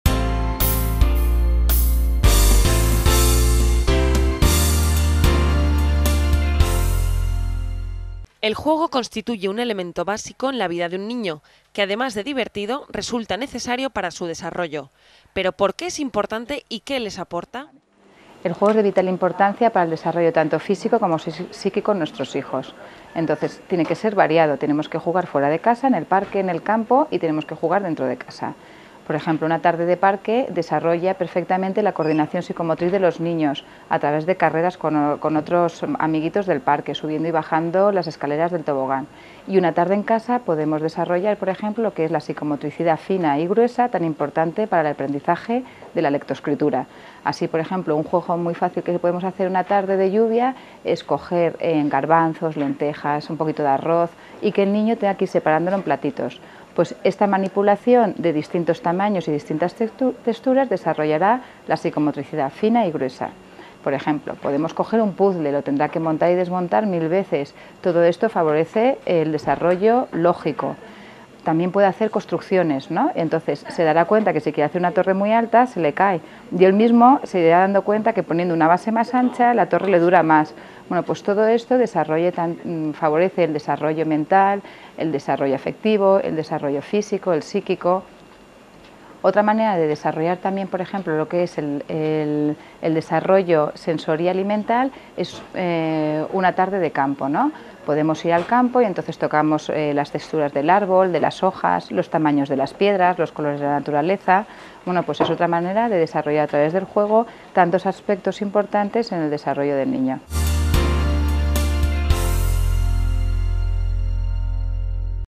El juego, un tema importante en la educación de un niño
El juego constituye un elemento básico en la vida de un niño, que además de divertido resulta necesario para su desarrollo. Pero ¿por qué es importante y qué les aporta?